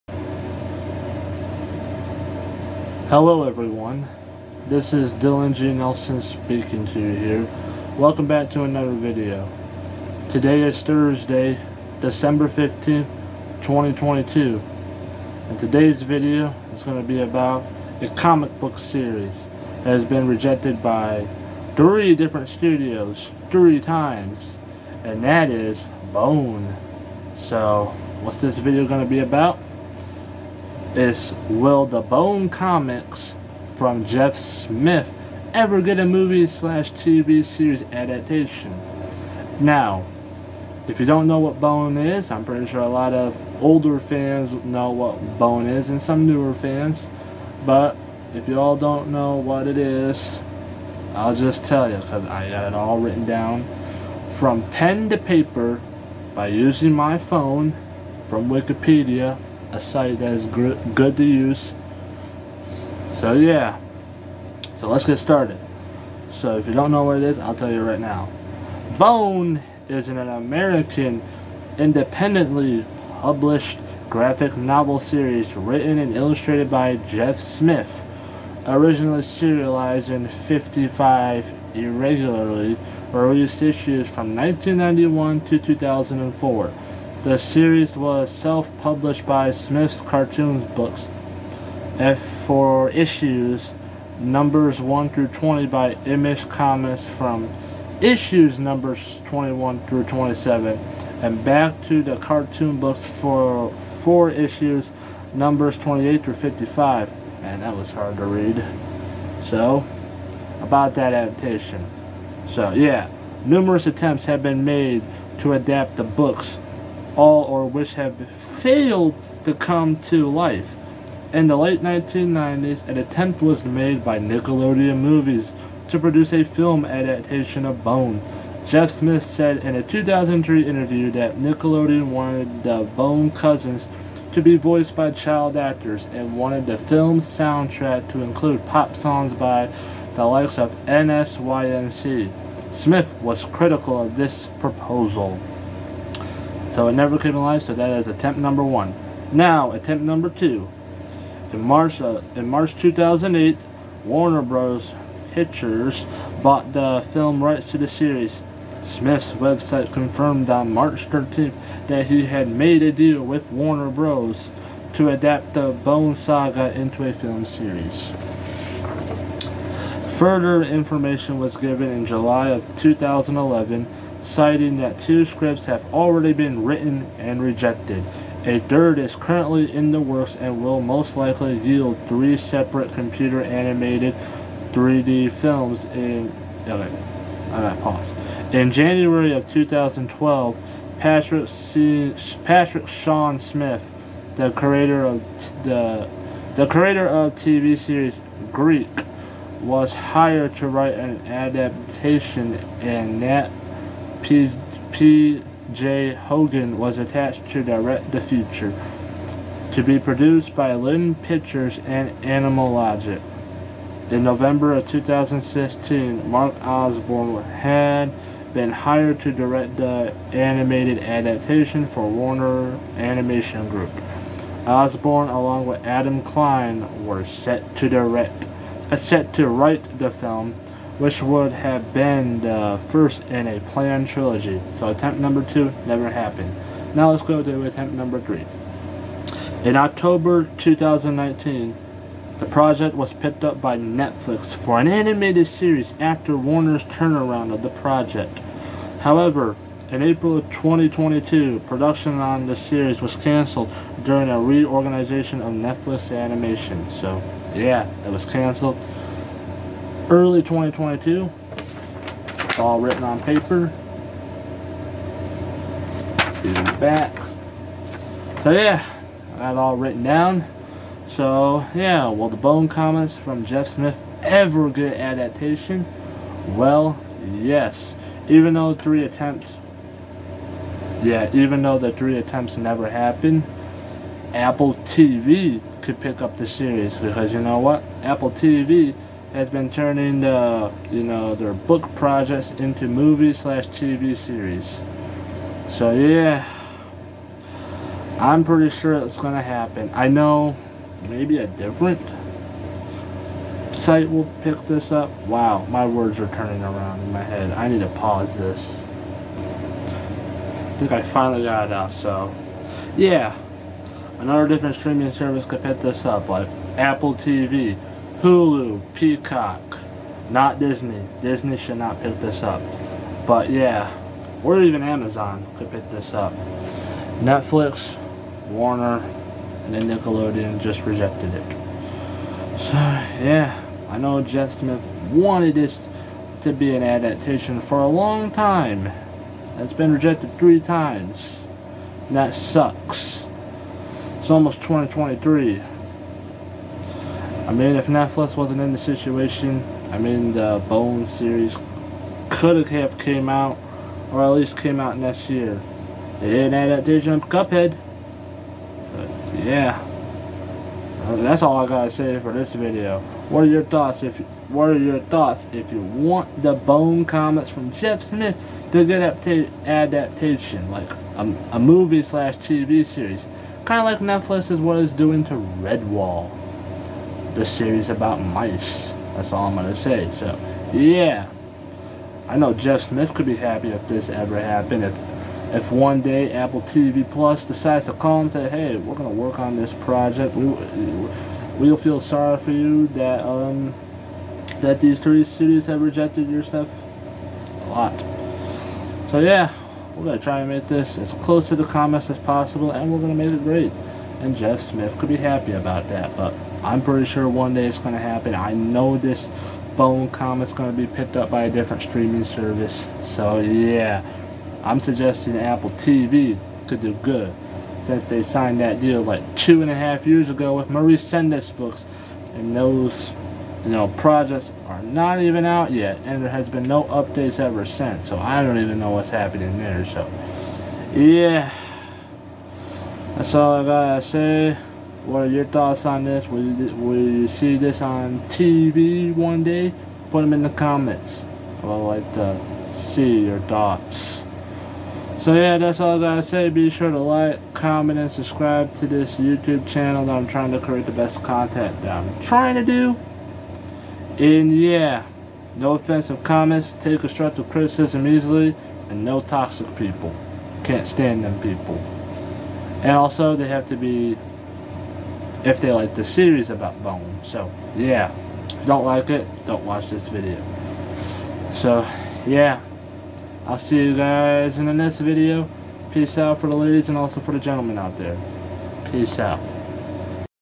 Will Jeff Smith's Bone Comic Series Ever A Get Movie/TV Series Adapation?
Will Jeff Smith's Bone Comic Book Series Ever Get A Movie/TV Series Adapation?